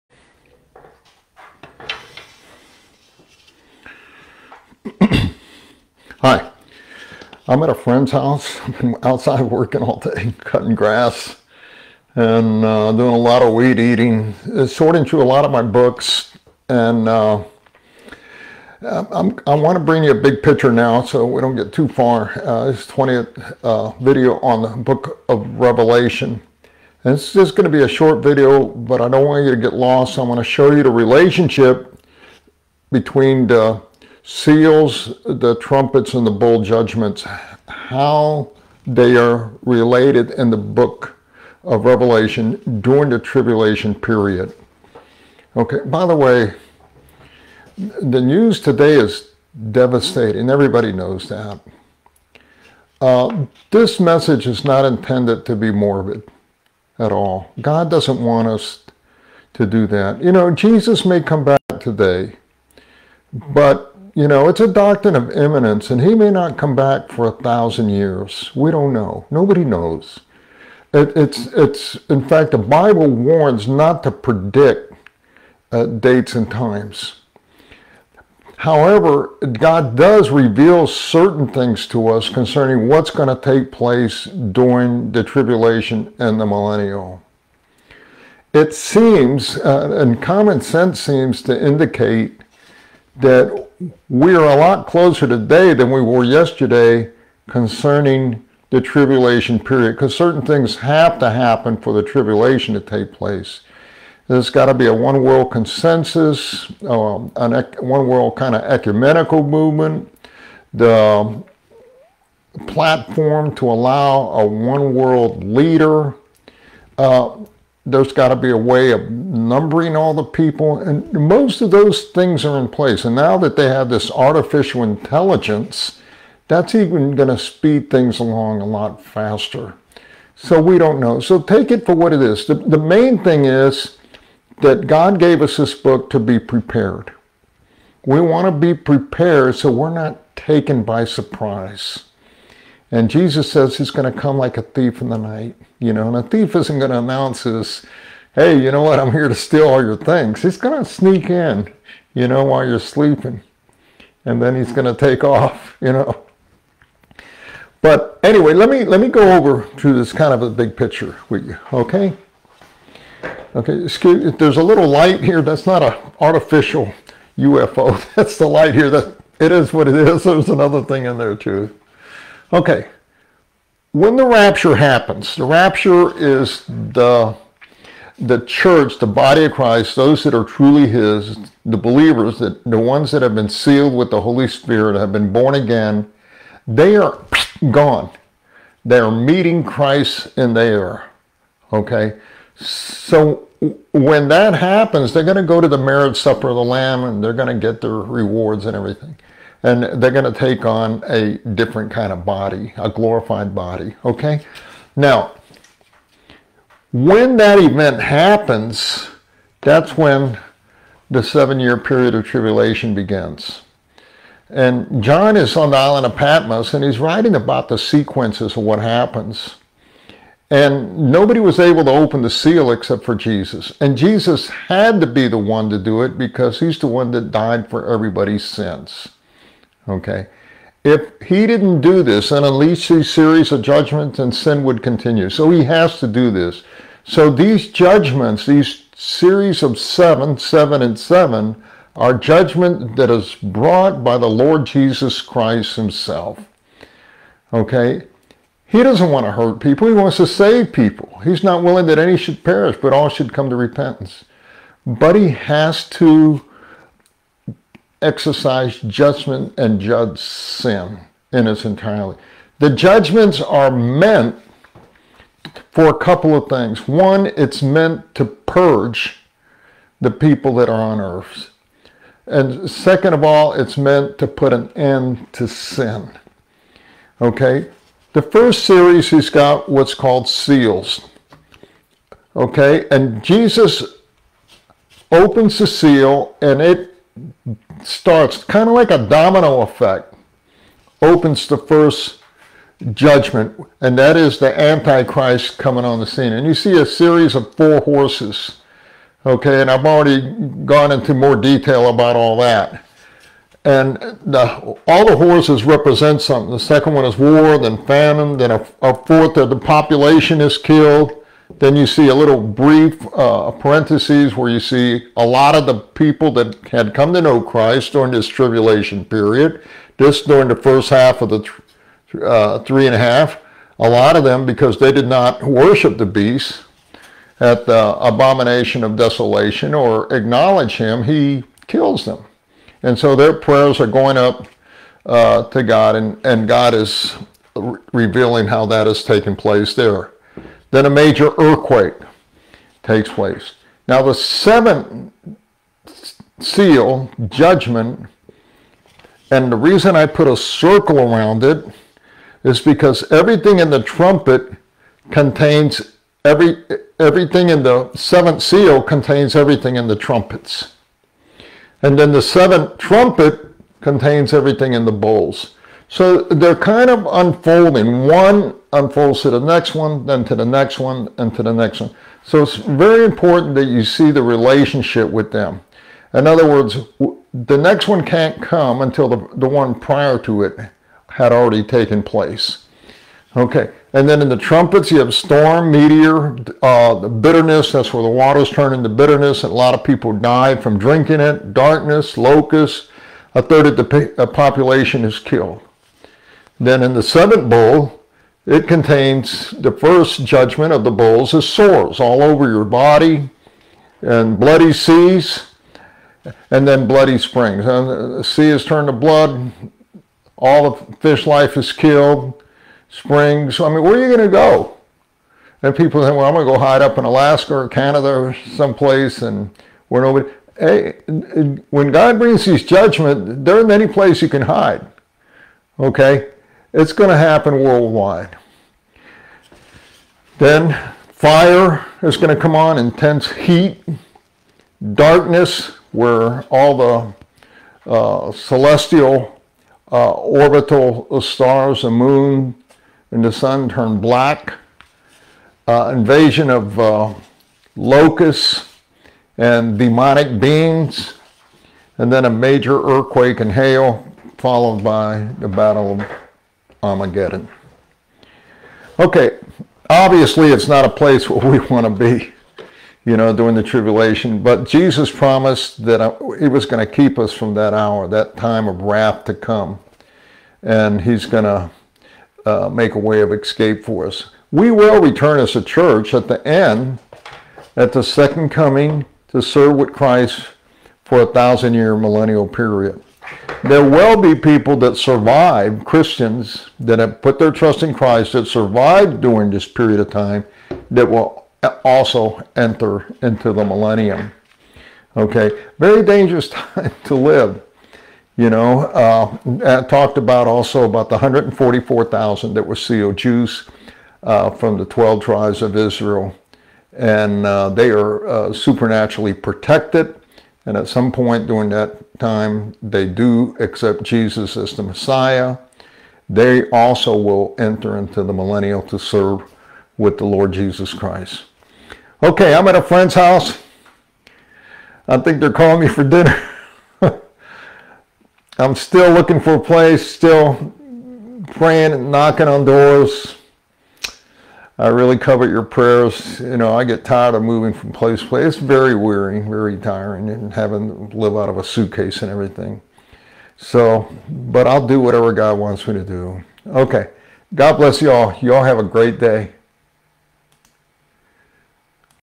#20 The Unfolding of Judgments
#20 Jesus unfolds the devastating Seals, Trumpets, and Bowel judgments on Earth.